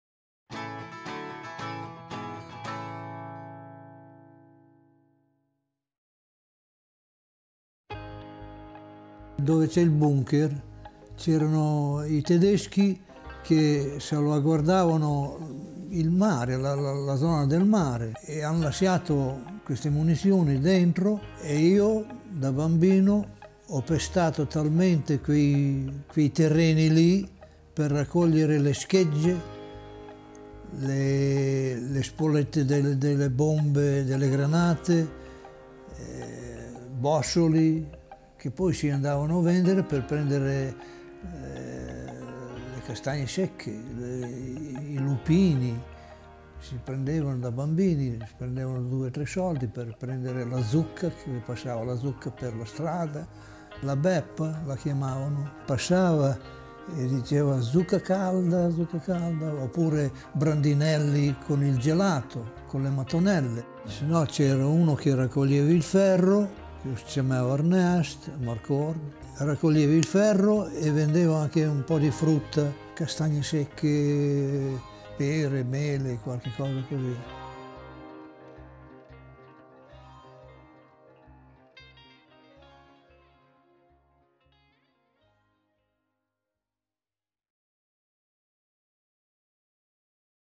Revival Interview || Cesenatico || Roberto Gozzoli about the bunker as children playground
In this interview Roberto Gozzoli tells about his memories as a child when the bunker was used as children playground